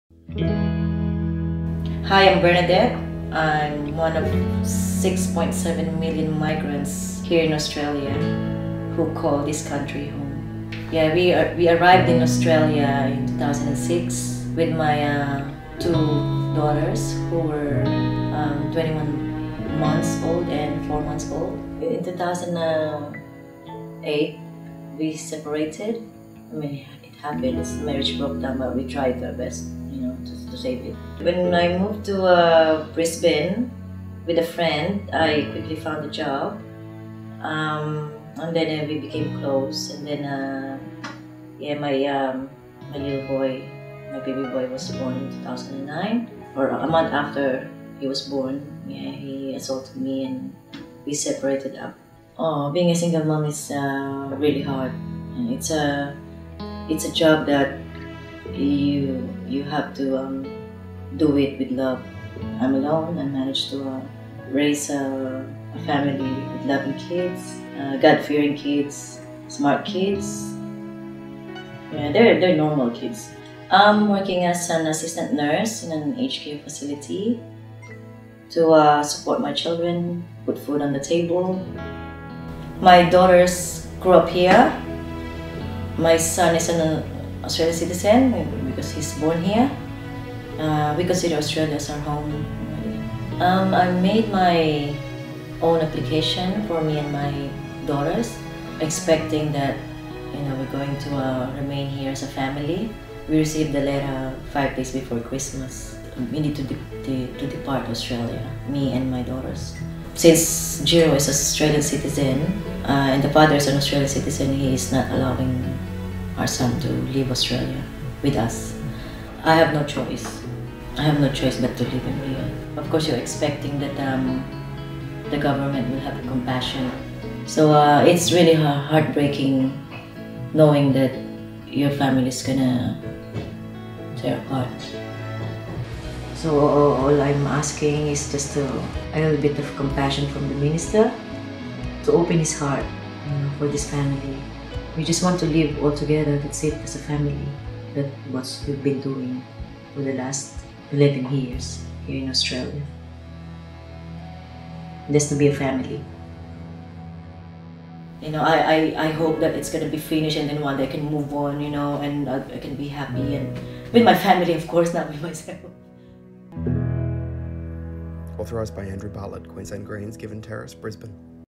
Bernadette's story
This is happening right here in Brisbane in 2018.

Bernadette, a mother of 3, will be made to leave Australia with her two teenage daughters. She will be forced to leave behind her 8 year old son who was born here. His Australian father has partial custody, so the boy can’t leave the country. The family will be separated permanently.

Australian law is meant to protect families. In cases like this, the minister responsible for immigration, Peter Dutton, can step in at any time to keep Bernadette’s family together. They have applied to become permanent residents for the last 6 years but the department keeps refusing then. Now time is running out.

Please sign the petition to ask the minister for Home Affairs to use his power to keep this family united here in Brisbane: